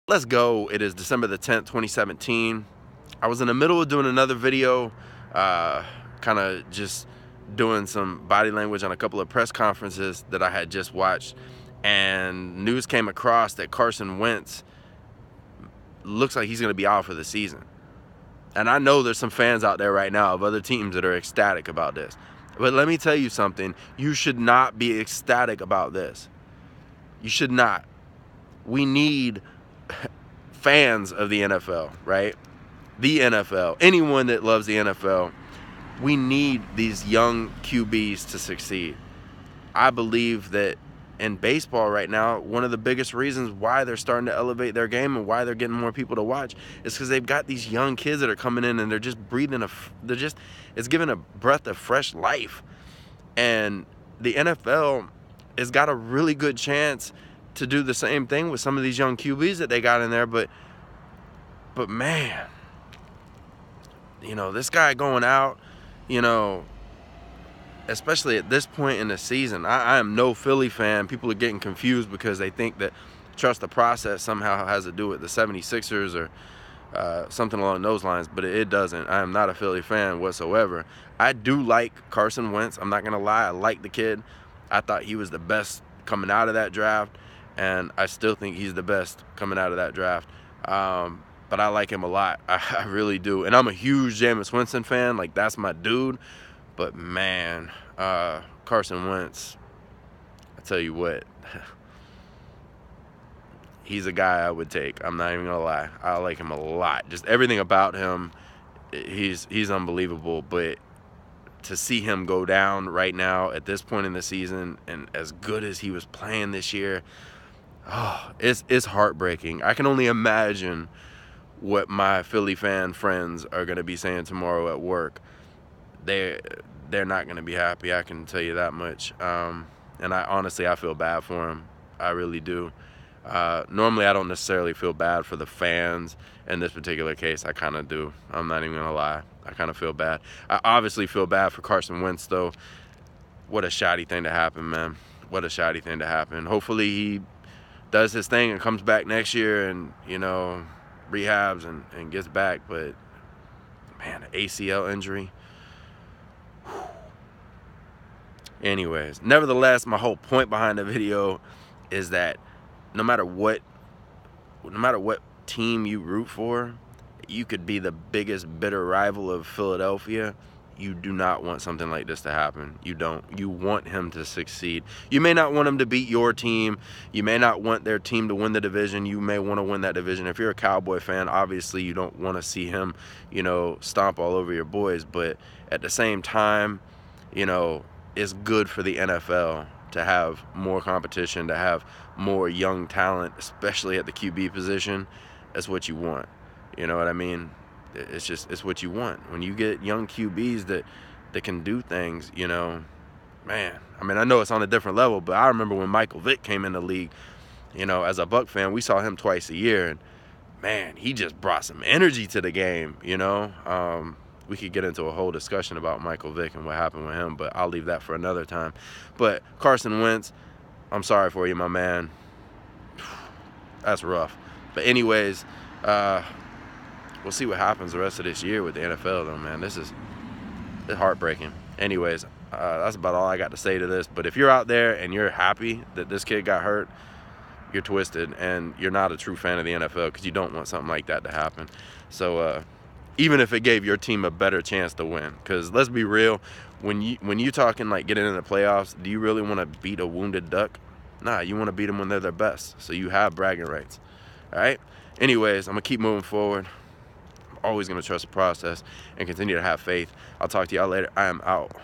Eagles Carson Wentz out for the Season and you're sick and twisted if you are happy about his
Some fans will actually be happy but I believe if you are a fan of the NFL you should want these young qb's to succeed